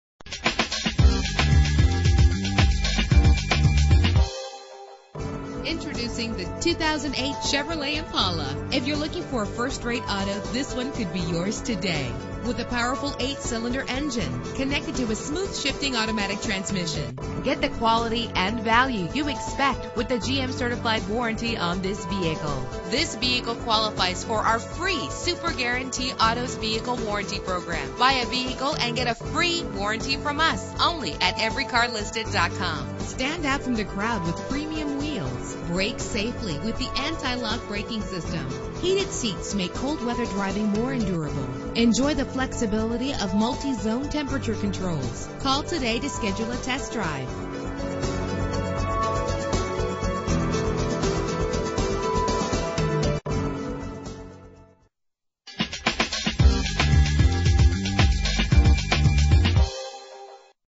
2008 Chevrolet Impala - Costa Mesa CA
Year: 2008
Make: Chevrolet
Model: Impala
Trim: SS
Engine: 5.3 liter 8 cylinder 16 valve
Transmission: 4-Speed Automatic
Color: Black
Mileage: 25570
Address:
2828 Harbor Blvd